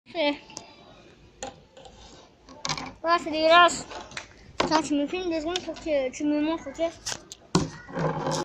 On joue dans Carrefour première partie
Enquête tous les déguisements d'Halloween de Carrefour City